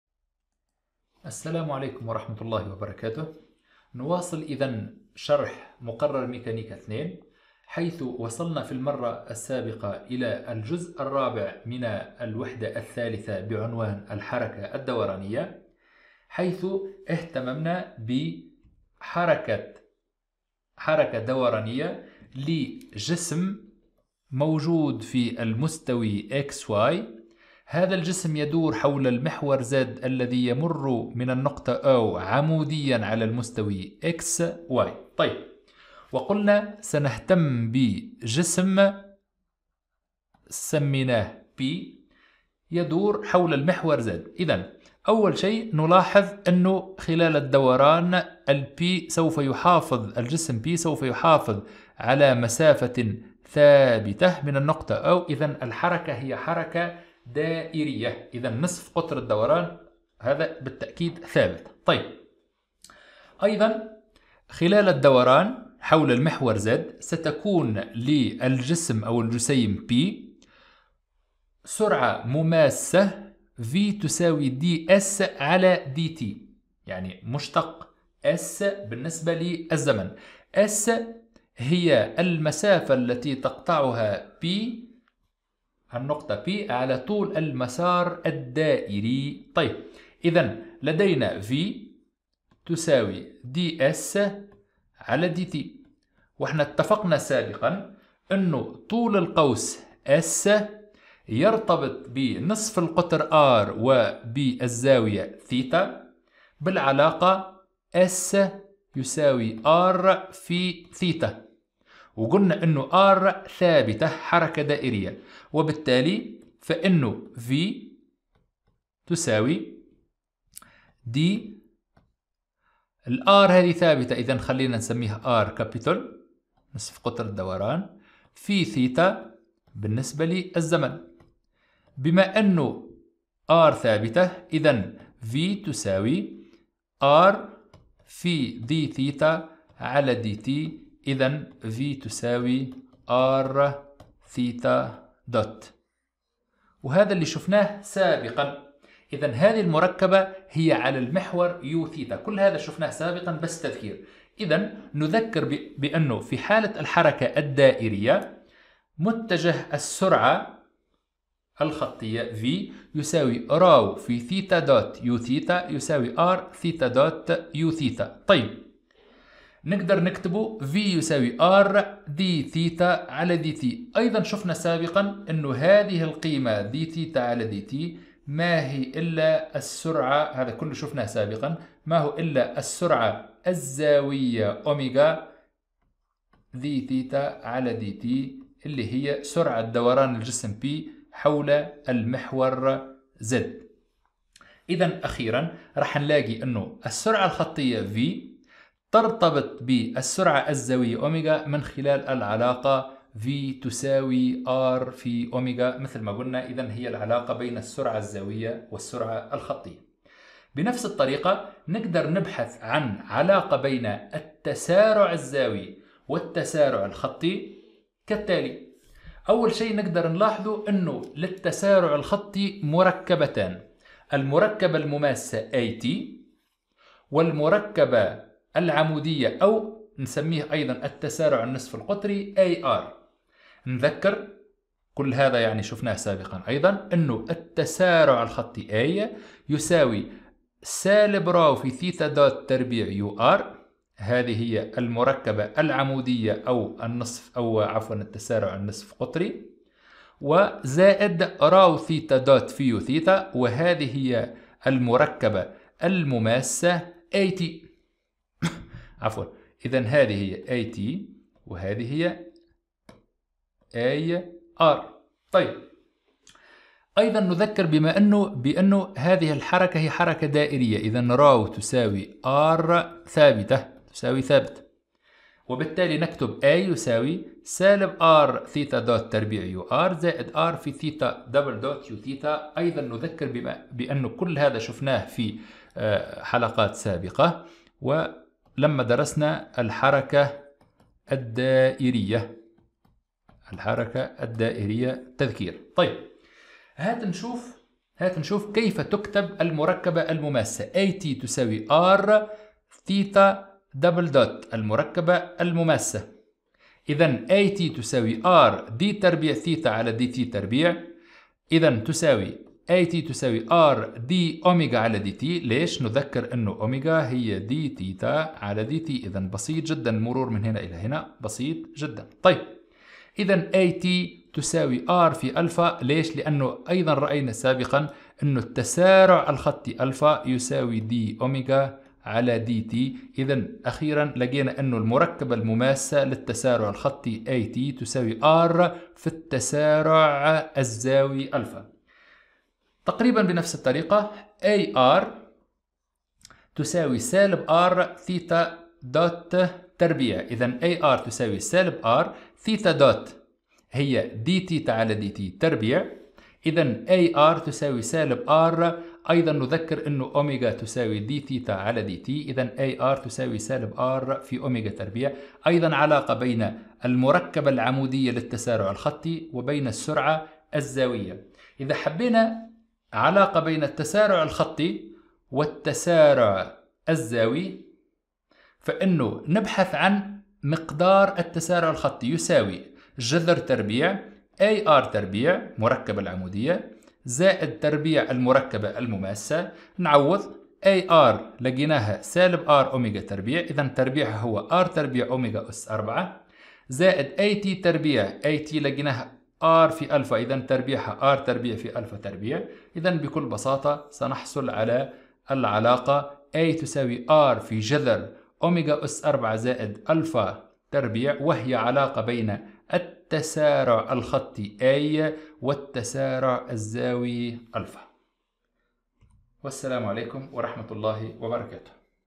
ميكانيكا 2 |3-5| الحركة الدورانية | العلاقة بين الكميات الخطية و الكميات الزاوية
المقررات المفتوحة - ميكانيكا - 2
الوحدة 3 :  الحركة الدورانية
الدرس 5 : العلاقة بين الكميات الخطية و الكميات الزاوية
• العلاقة بين السرعة الخطية و السرعة الزاوية
• العلاقة بين التسارع الخطي و التسارع الزاوي